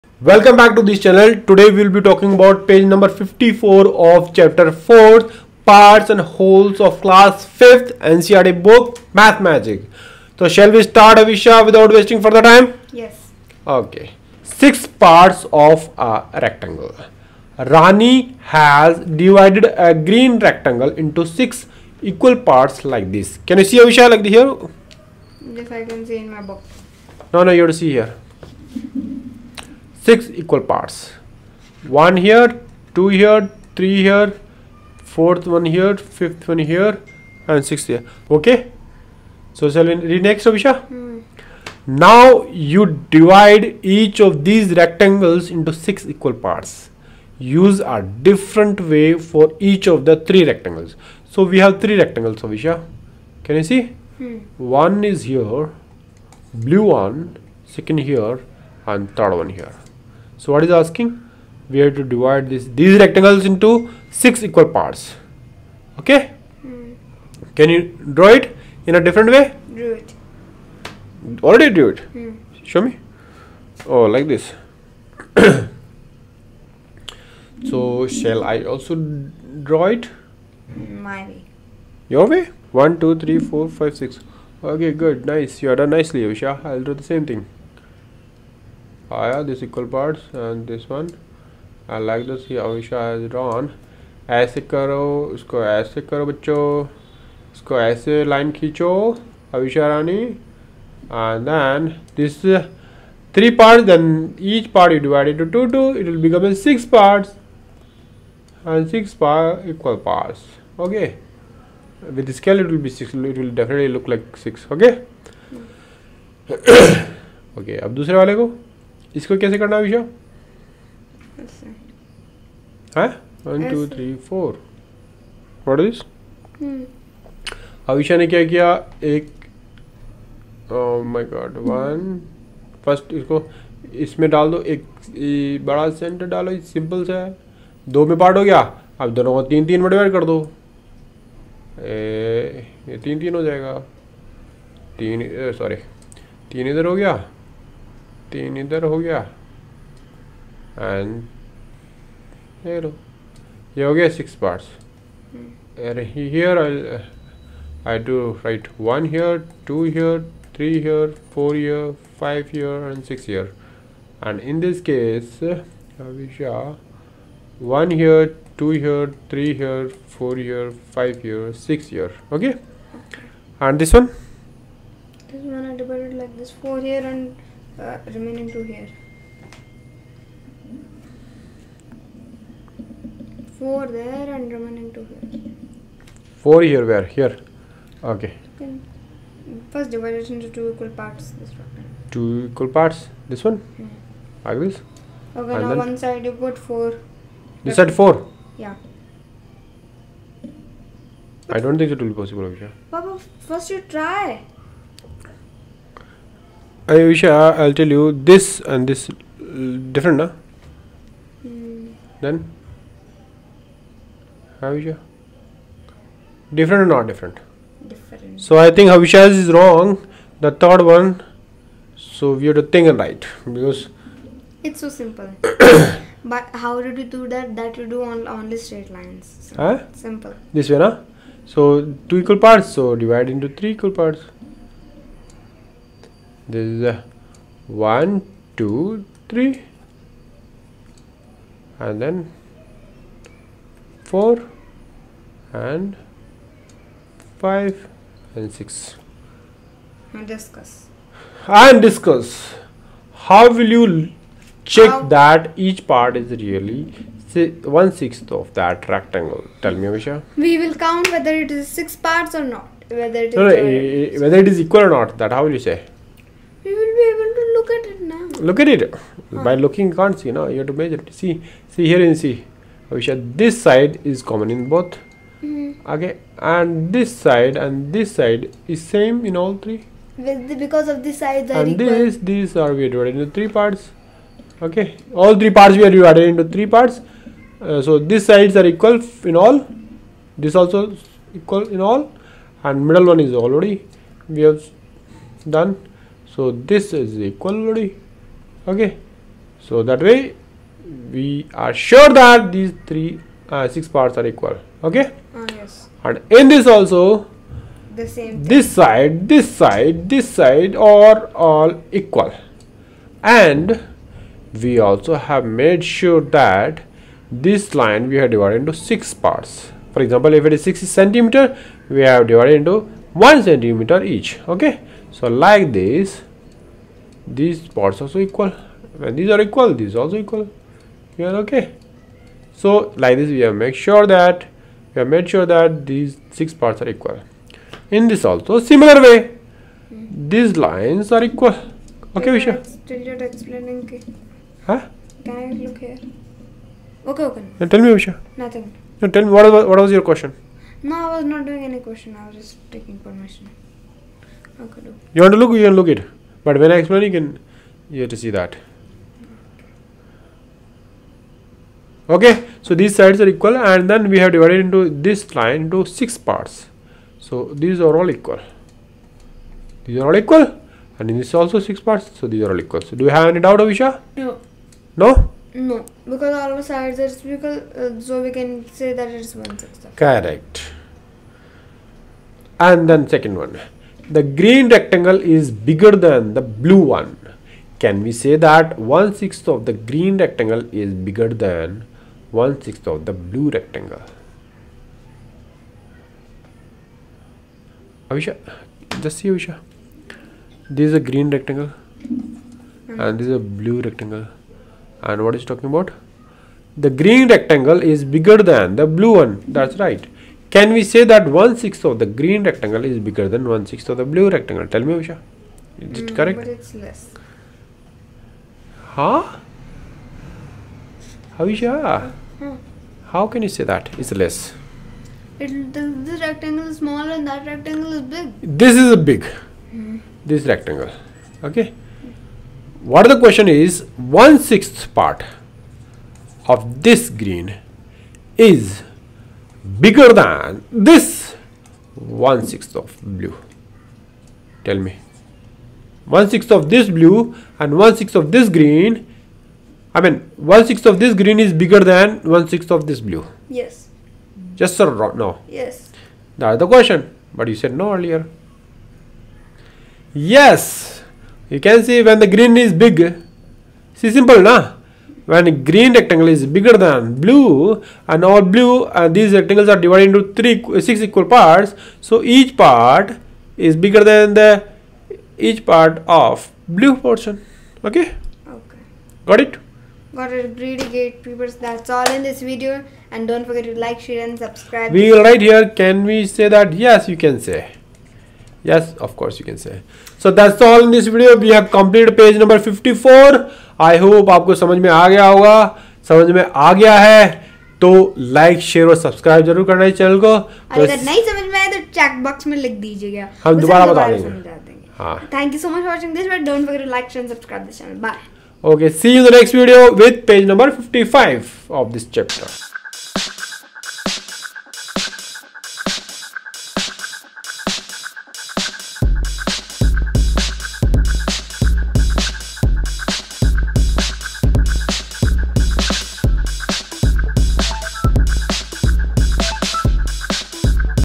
NCERT Class 5 Maths Chapter 4 II Parts and Wholes II Page 54 II Math Magic
This video explains in detail chapter 4 Parts and Wholes ? Page no. 54 of Maths (Math Magic) of Class 5. I encourage you to watch this video completely.